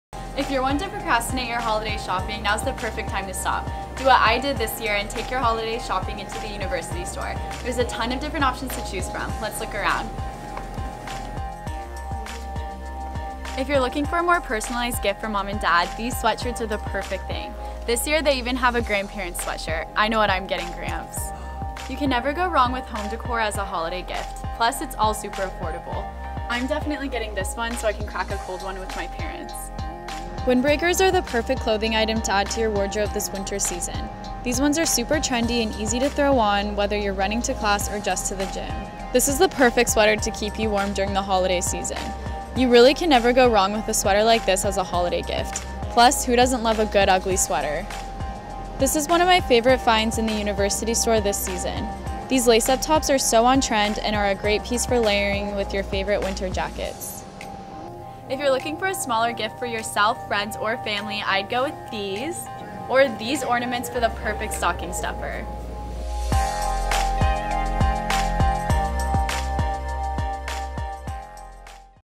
Holiday Gift Guide 2017 at the University Store
Find out what gifts to get your whole family for the holidays. For more information on products, check out the Sonoma State University's Store Holiday Collection here:
and see what items are on sale including cold weather and fleece until December 15, 2017

Make sure to check us out on all platforms so you're always in the know of what's going on at SSU!